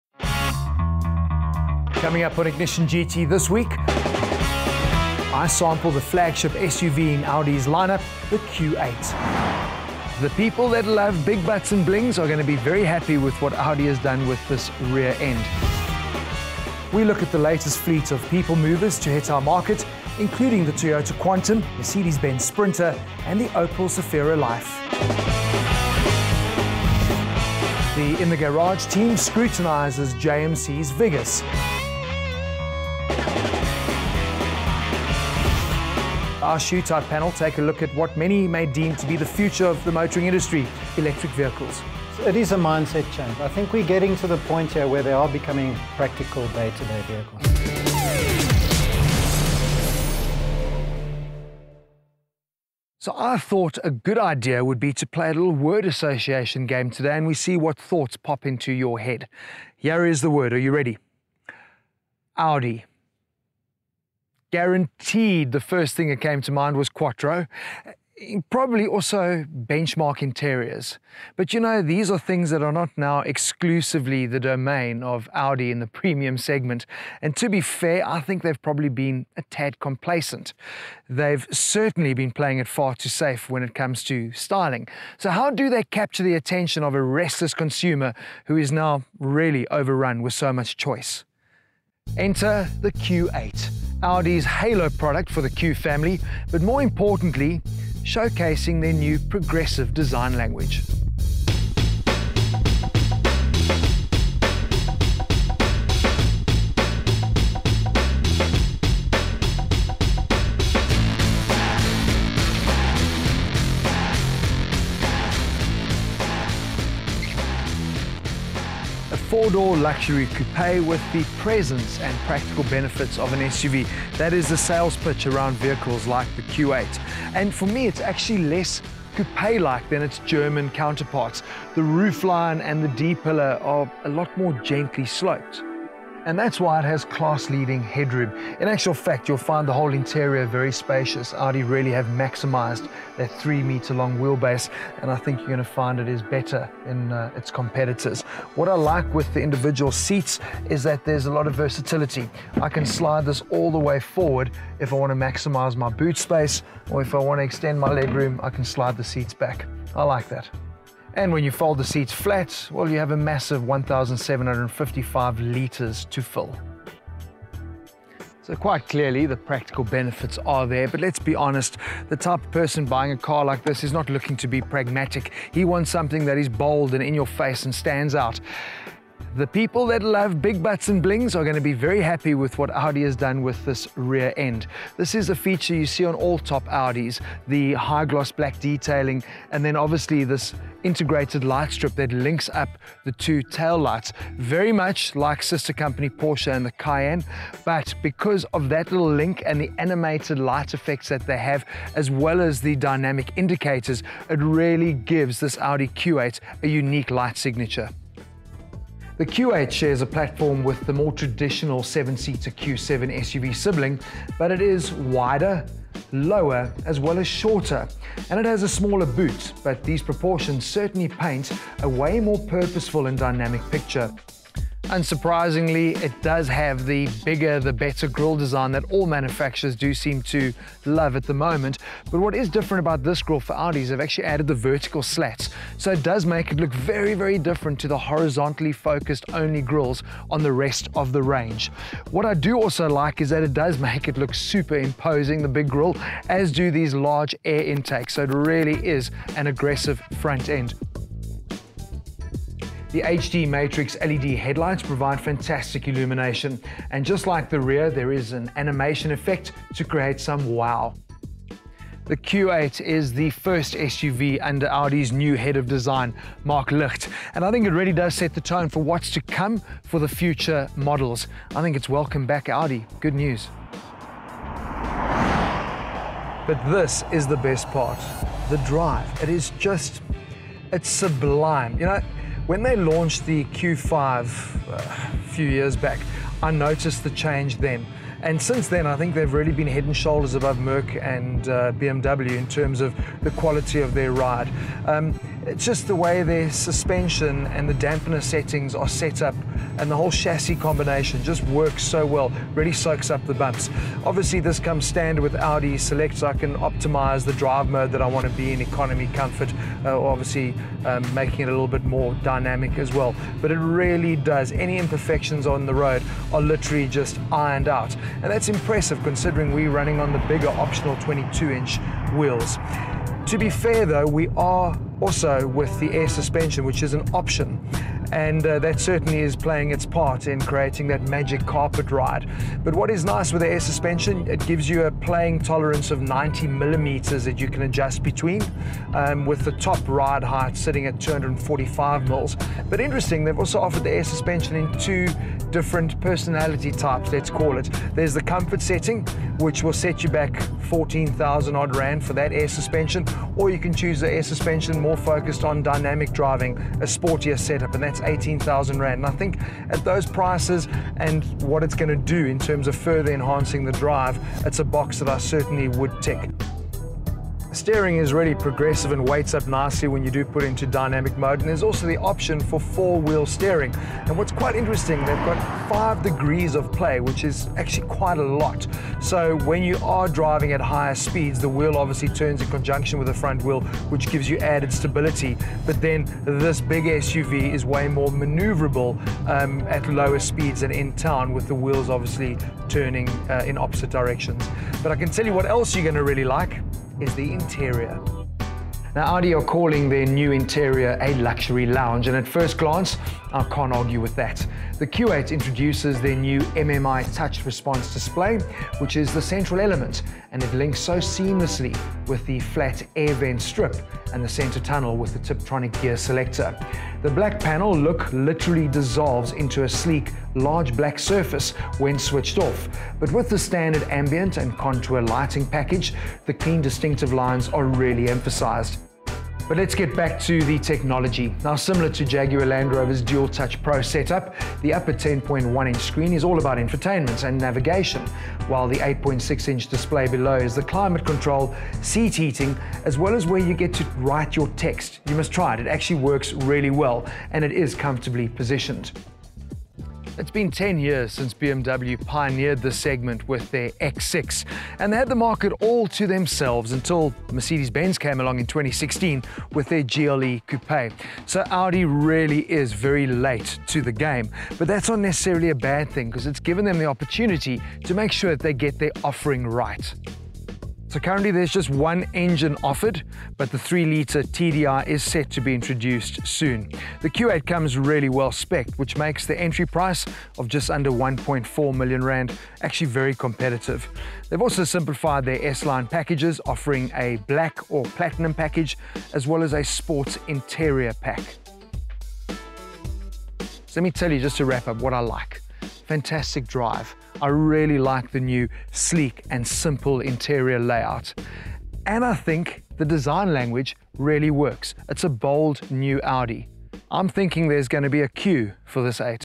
Audi Q8 Review
Marius gets into the drivers seat of Audi's newest offering in the premium SUV space, the Q8